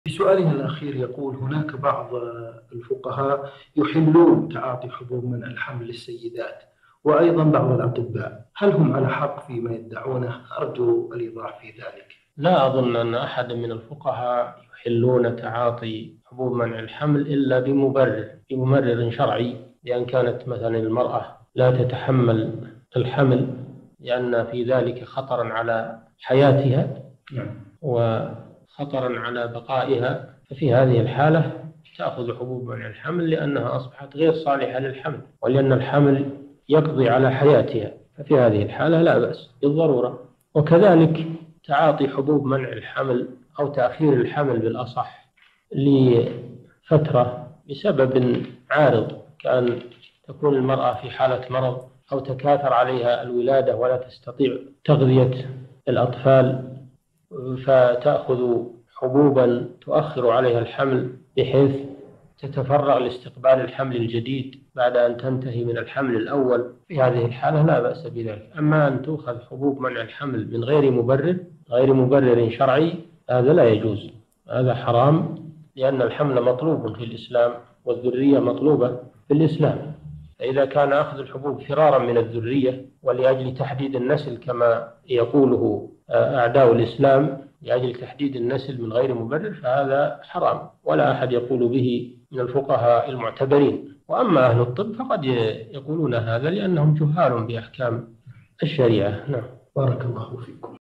هناك بعض الفقهاء والأطباء يحلون تعاطي حبوب منع الحمل للسيدات هل هم على حق في ذلك؟ الشيخ صالح الفوزان
نص السؤال:
هناك بعض الفقهاء يحلون تعاطي حبوب منع الحمل للسيدات وأيضاً بعض الأطباء هل هم على حق في ذلك أرجو الإيضاح في ذلك؟

السيرة الذاتية :
للاشتراك في قناة الشيخ صالح الفوزان

رئيس الفريق
لمشروع كبار العلماء